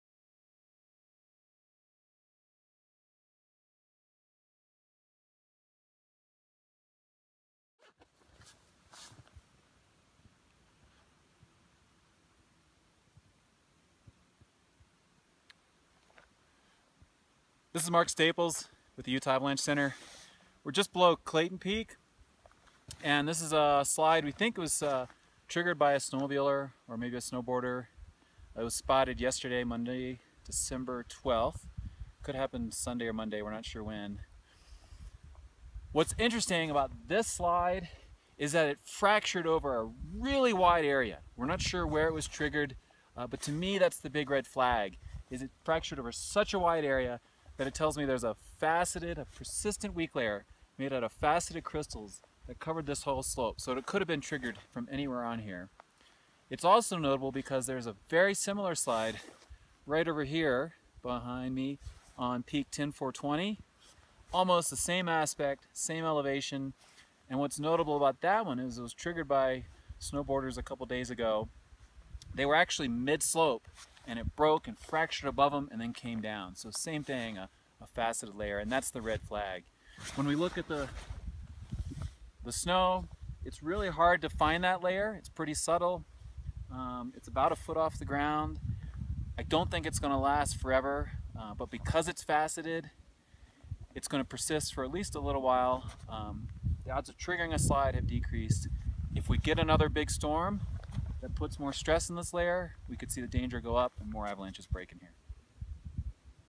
Avalanche on Clayton Pk - 13 Dec 2016
Mark Staples of the UAC looks at an avalanche that likely happened Sunday, Dec 11. It broke on a thin layer of small facets about a foot above the ground. The avalanche was 2ft deep, 250ft wide, and 400ft vertical.